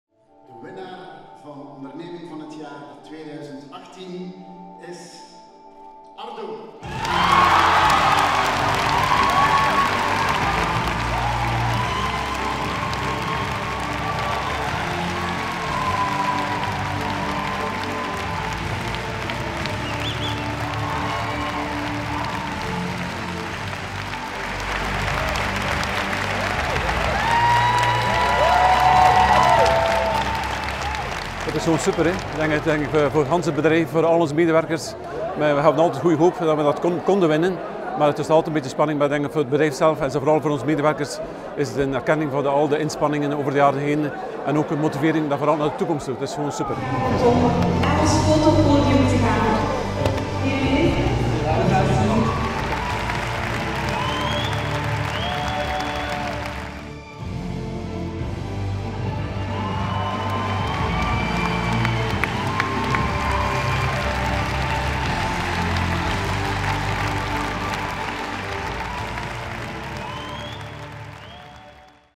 Ardo is Onderneming van het Jaar 2018
Ardo, de West-Vlaamse producent van diepvriesgroenten, sleept de onderscheiding 'Onderneming van het Jaar 2018’ in de wacht en volgt daarmee de magazijnspecialist WDP op.